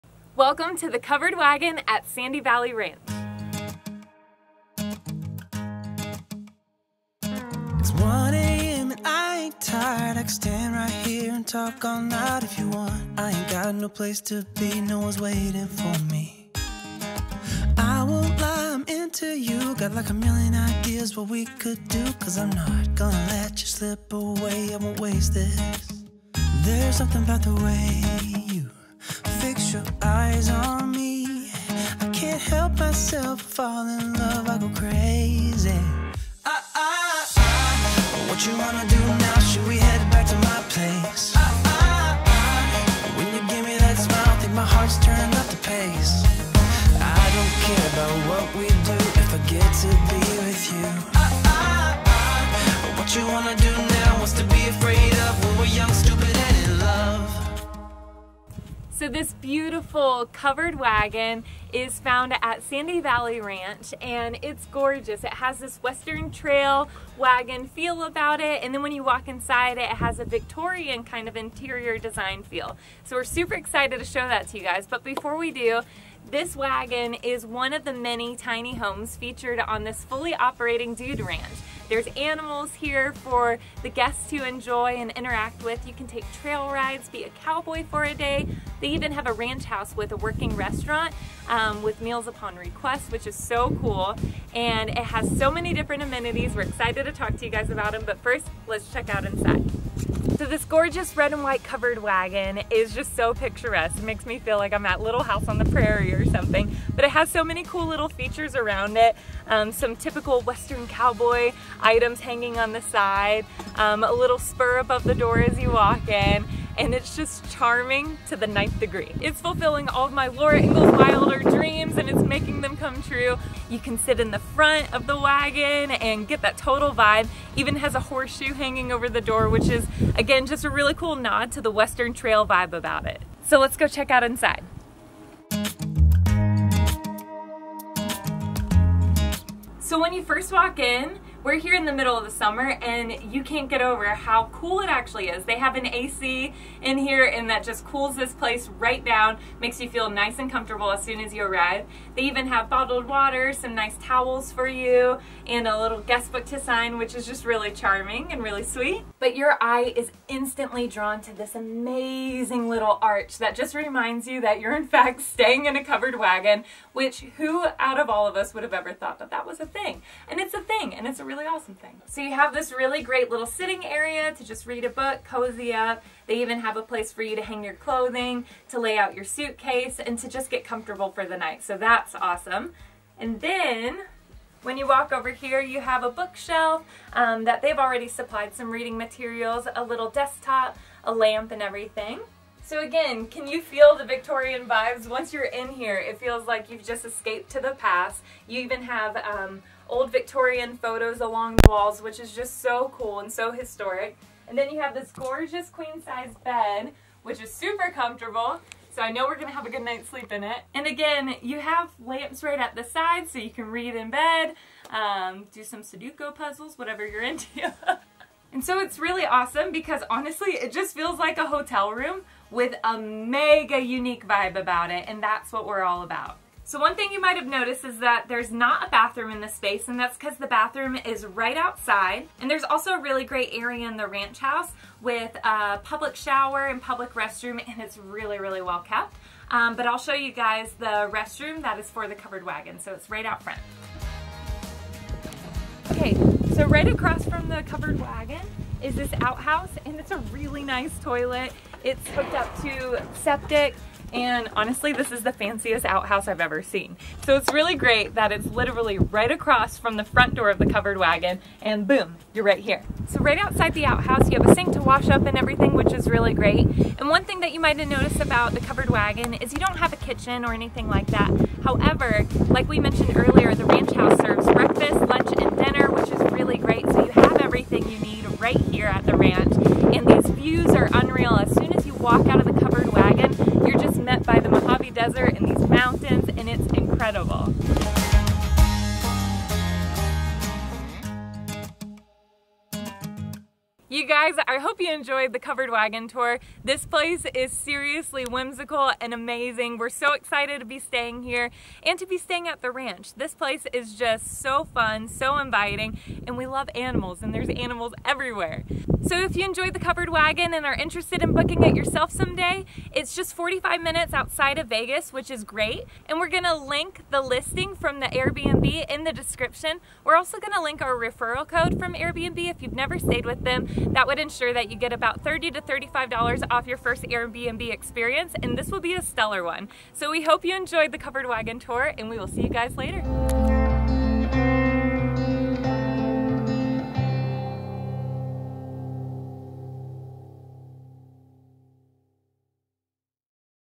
Covered Wagon | AirBnb Full Tour | Tiny House
Here is a full tour of the Covered Wagon Tiny House listed on AirBnb at Sandy Valley Ranch, Nevada.

Rep our Hive:
B-Shirts |Bonfire Store: